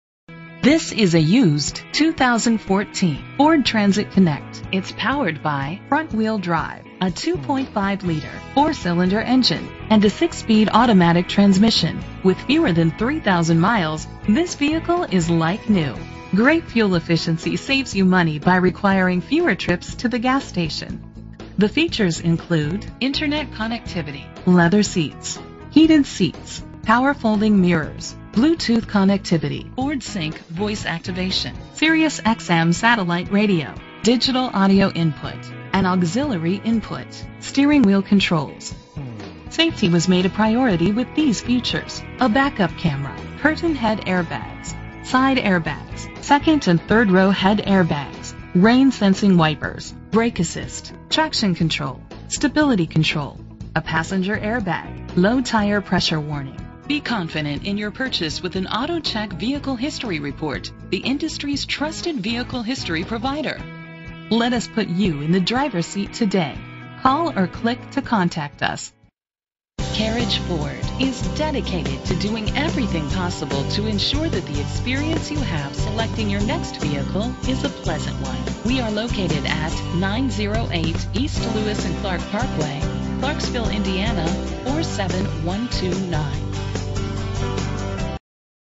2014 Ford Transit Connect - Clarksville IN
Phone:
Year: 2014
Make: Ford
Model: Transit Connect
Trim: Titanium
Engine: 2.5 liter inline 4 cylinder 16 valve
Transmission: 6-Speed Automatic
Color: Tectonic Silver
Mileage: 2043
Address:
908 E. Highway 131